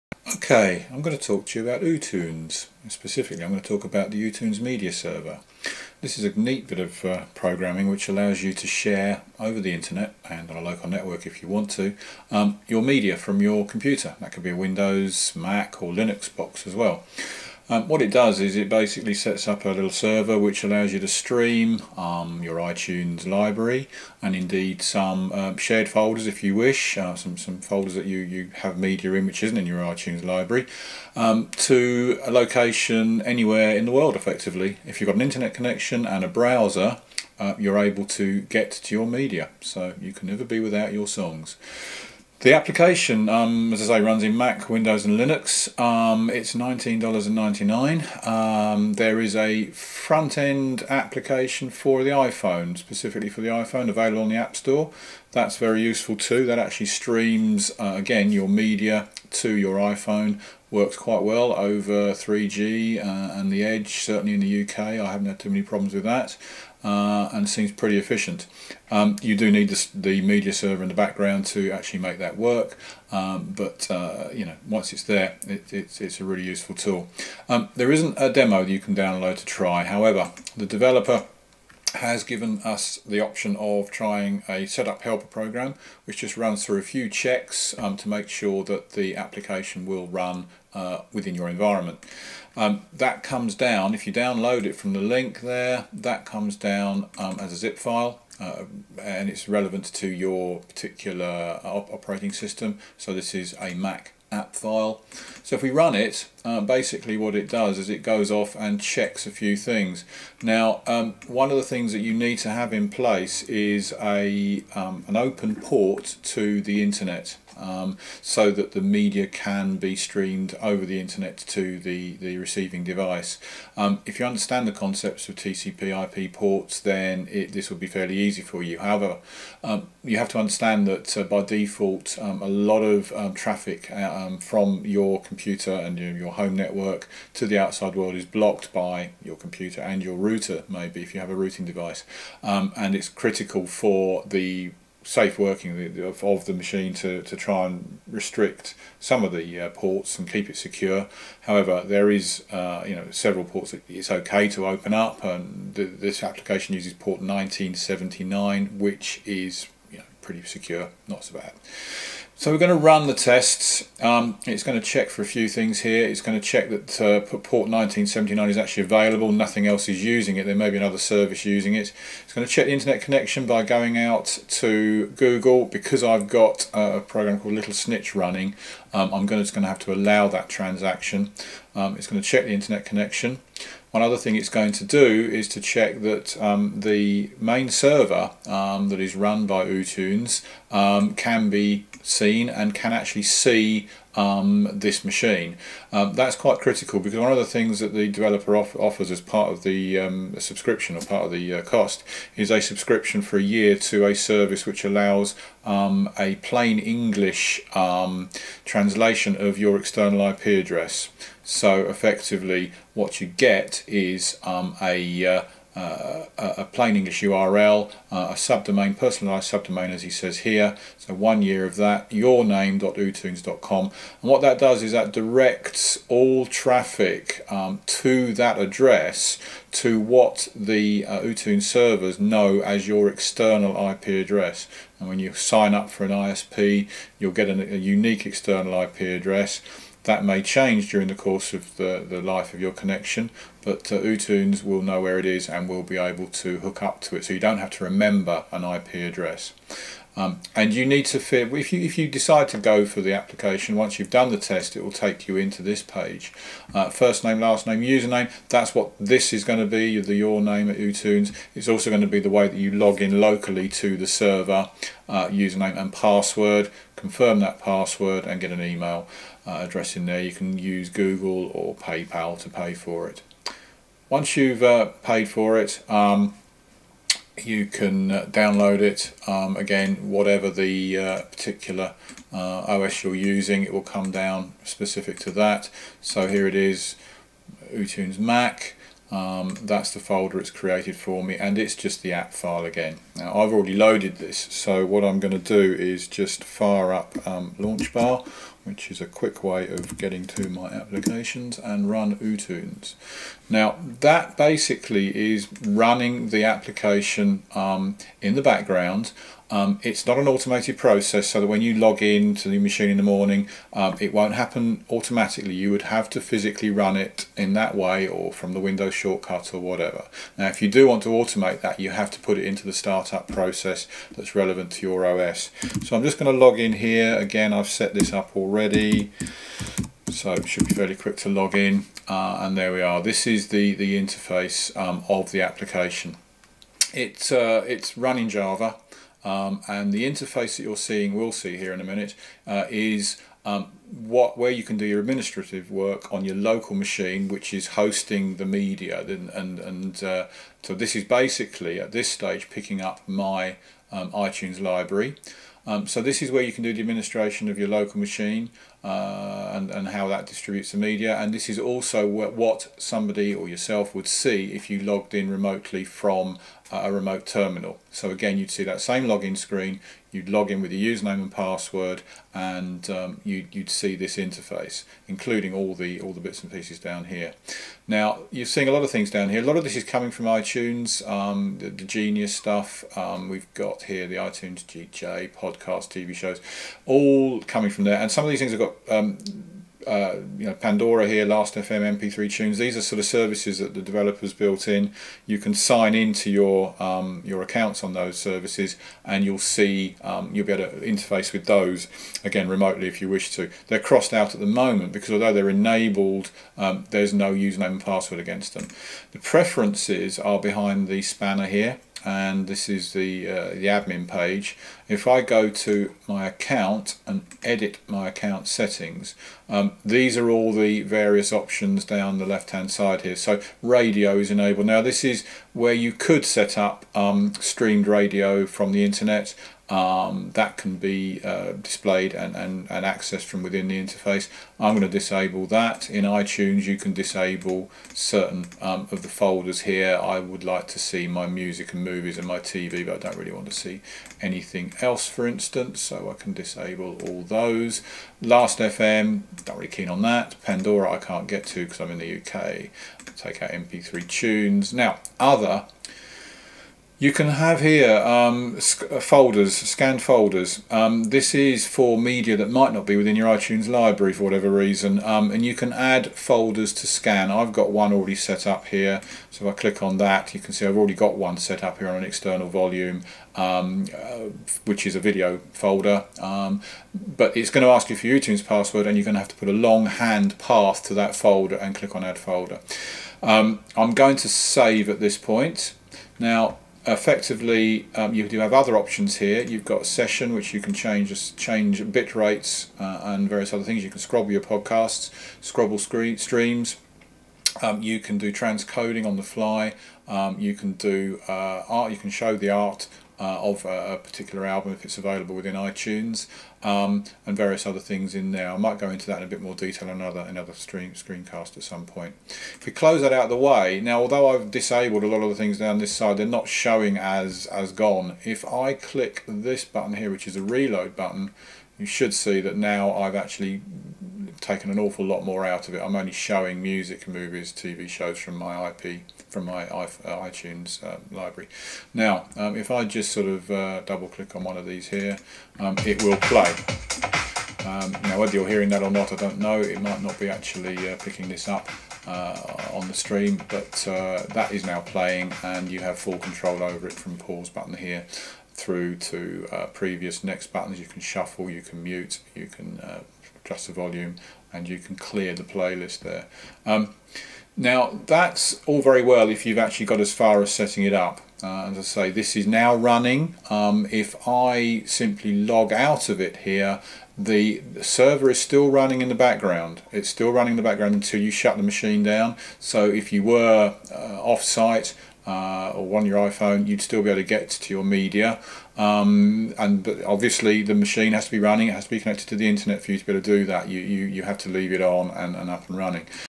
Introduction to ooTunes personal media server...
ooTunes media server is a great way to access your music and videos from the Internet - even on an iPhone. The server is available for Mac, Windows or Linux and this video describes the set up and some of the options available in the application.